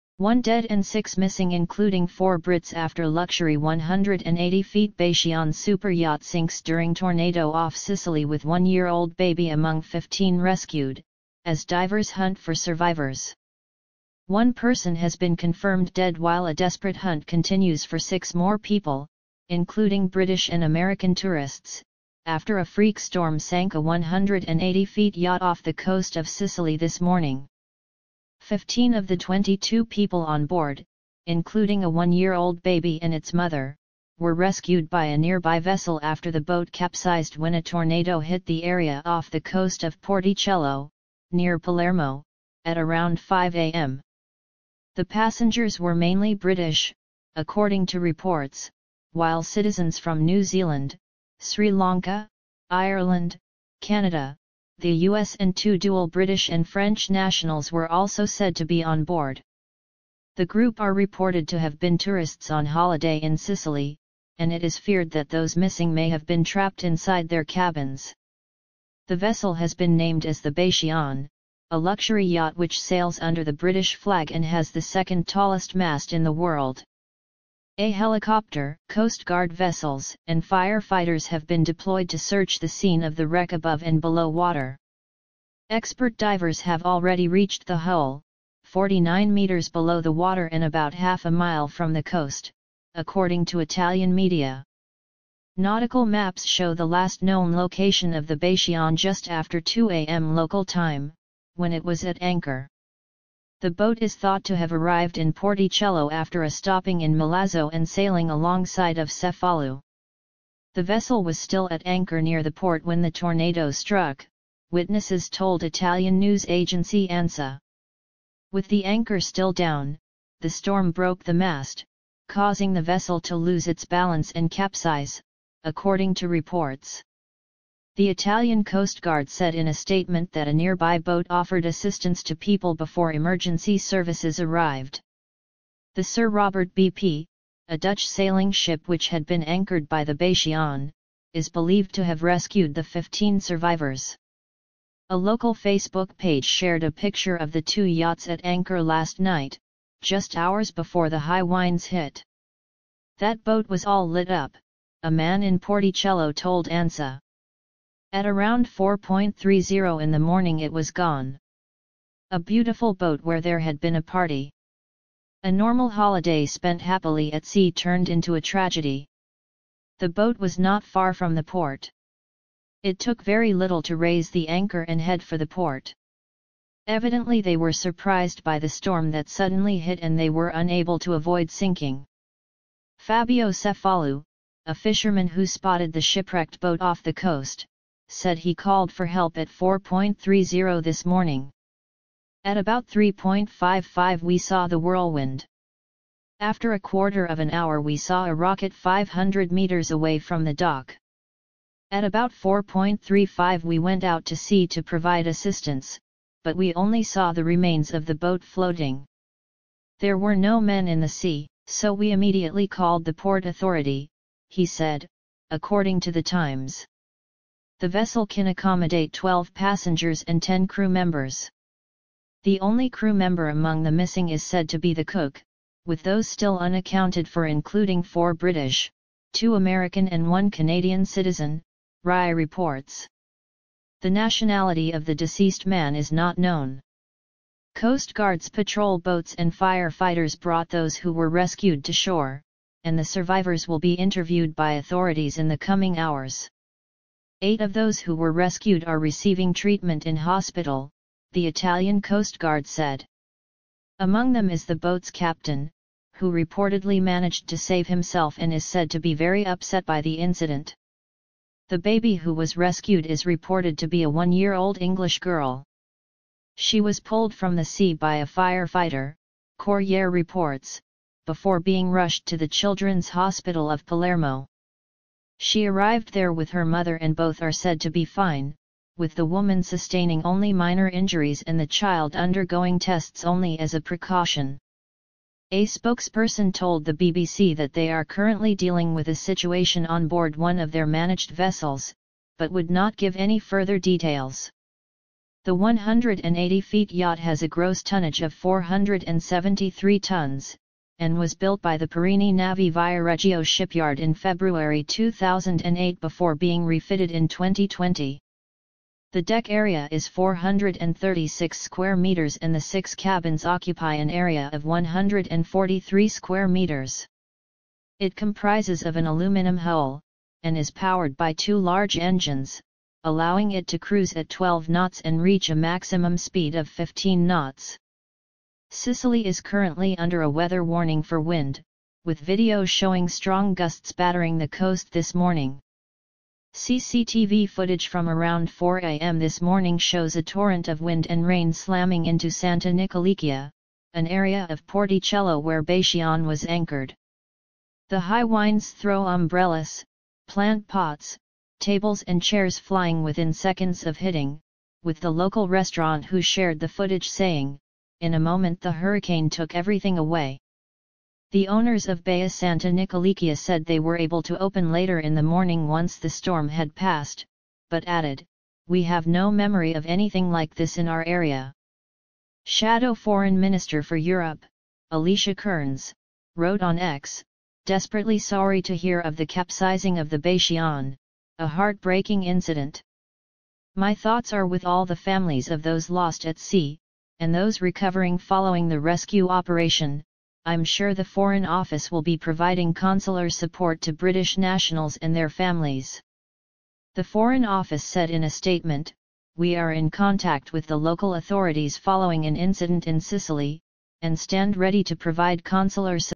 One dead and six missing including four Brits after luxury 180ft Bayesianyatch sinks
One dead and six missing including four Brits after luxury 180ft Bayesian superyacht sinks during tornado off Sicily with one-year-old baby among 15 rescued - as divers hunt for survivors

British tech entrepreneur Mikelynch missing